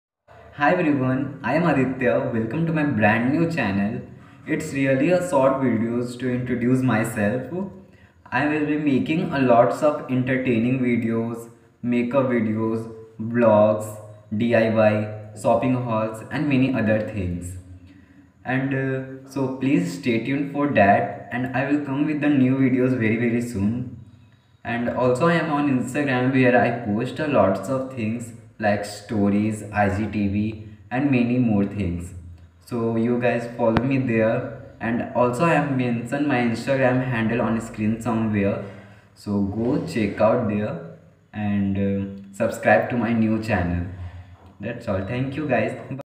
MY FIRST YOUTUBE VIDEO😋|| Introducing my youtube channel
This video is about a short introduction to my channel. I like making content that entartains you & hopefully puts a smile on your face. I'll try uploading fun stuff as regularly and consistently possible.

Okay byeeeee.    :)